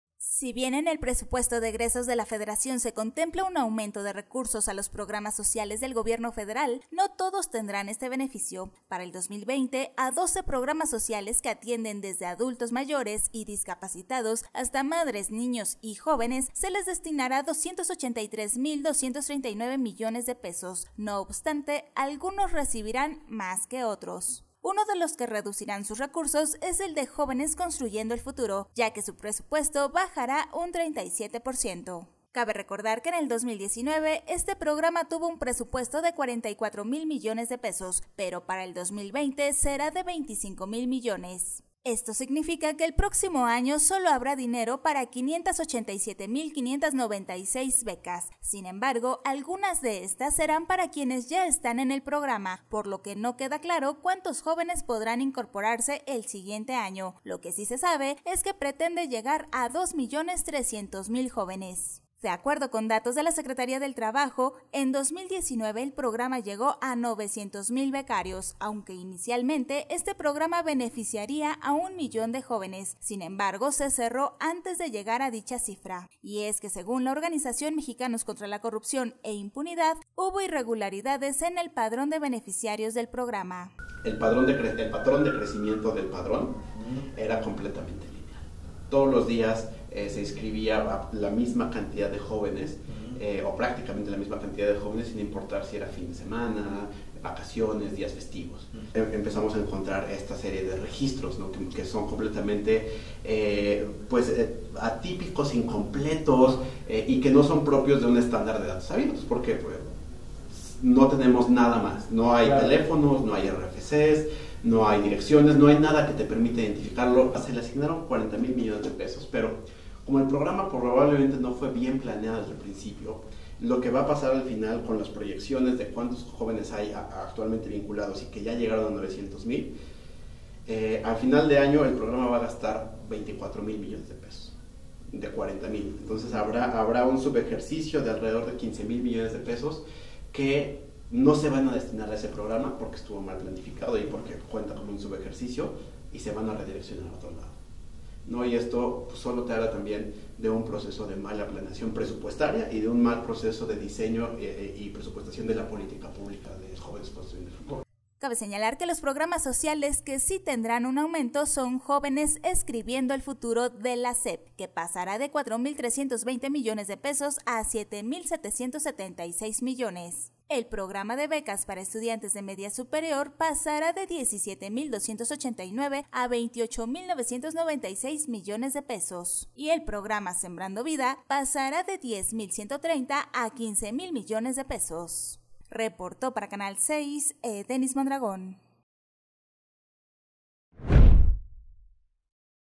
Se recortó el presupuesto del programa Jóvenes Construyendo el Futuro
Nacional | Se recortó el presupuesto del programa Jóvenes Construyendo el Futuro